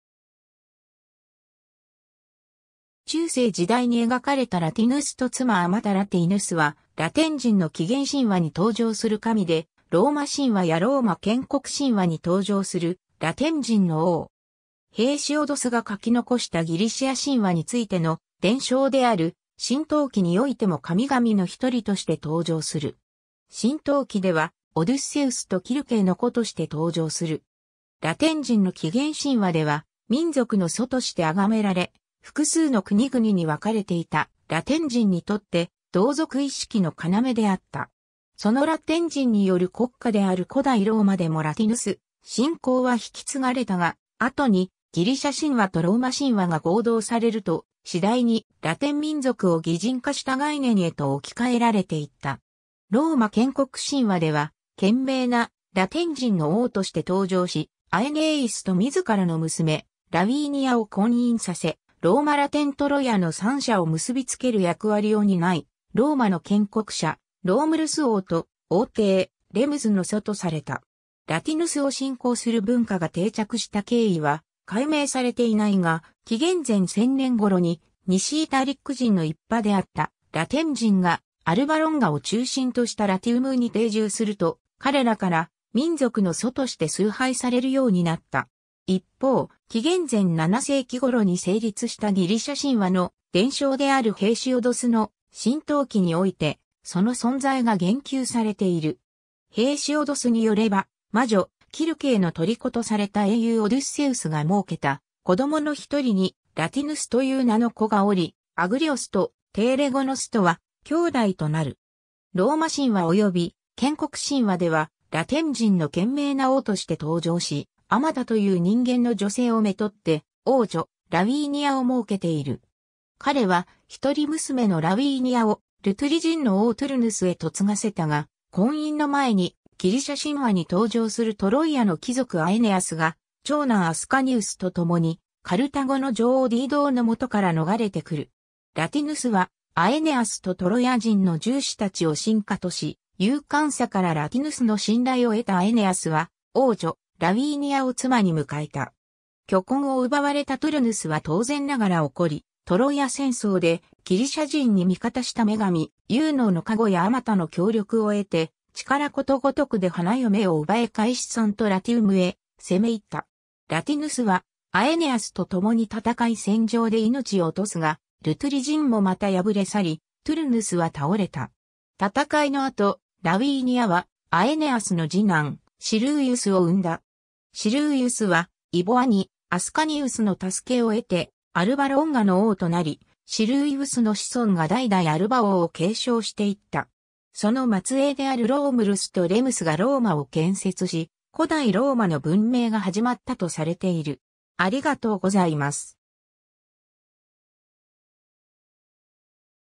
ラティヌス
神話・伝説の統治者
中世時代に描かれたラティヌスと妻アマタ ラティヌス（Lătīnŭs）はラテン人の起源神話に登場する神で、ローマ神話やローマ建国神話に登場するラテン人の王。
 ヘーシオドスが書き残したギリシア神話についての伝承である『神統記』においても神々の一人として登場する。
 『神統記』ではオデュッセウスとキルケーの子として登場する。
ラテン人の起源神話では民族の祖として崇められ、複数の国々に分かれていたラテン人にとって同族意識の要であった。
そのラテン人による国家である古代ローマでもラティヌス信仰は引き継がれたが、後にギリシャ神話とローマ神話が合同されると（ギリシャ・ローマ神話）、次第にラテン民族を擬人化した概念へと置き換えられていった。
 ローマ建国神話では賢明なラテン人の王として登場し、アエネーイスと自らの娘ラウィーニアを婚姻させ、ローマ・ラテン・トロイアの三者を結びつける役割を担い、ローマの建国者ロームルス王と王弟レムスの祖とされた。
 ラティヌスを信仰する文化が定着した経緯は解明されていないが、紀元前1000年頃に西イタリック人の一派であったラテン人がアルバ・ロンガを中心としたラティウムに定住すると、彼らから民族の祖として崇拝されるようになった。
一方、紀元前7世紀頃に成立したギリシャ神話の伝承であるヘーシオドスの『神統記』において、その存在が言及されている。
ヘーシオドスによれば、魔女キルケーの虜とされた英雄オデュッセウスが儲けた子供の一人にラティヌスという名の子がおり、アグリオスとテーレゴノスとは兄弟となる。
 ローマ神話及び建国神話ではラテン人の賢明な王として登場し、アマタという人間の女性を娶って王女ラウィーニアを儲けている。
彼は一人娘のラウィーニアをルトゥリ人の王トゥルヌスへ嫁がせたが、婚姻の前にギリシャ神話に登場するトロイアの貴族アエネアスが長男アスカニウスと共に、カルタゴの女王ディードーの元から逃れてくる。
ラティヌスはアエネアスとトロイア人の従士達を臣下とし、勇敢さからラティヌスの信頼を得たアエネアスは王女ラウィーニアを妻に迎えた。
 許婚を奪われたトゥルヌスは当然ながら怒り、トロイア戦争でギリシャ人に味方した女神ユーノーの加護やアマタの協力を得て、力尽くで花嫁を奪え返さんとラティウムへ攻め入った。
ラティヌスはアエネアスと共に戦い戦場で命を落とすが、ルトゥリ人もまた敗れ去り、トゥルヌスは倒れた。
戦いの後、ラウィーニアはアエネアスの次男シルウィウスを生んだ。
シルウィウスは異母兄アスカニウスの助けを得てアルバ・ロンガの王（アルバ王）となり、シルウィウスの子孫が代々アルバ王を継承していった。
その末裔であるロームルスとレムスがローマを建設し、古代ローマの文明が始まったとされている。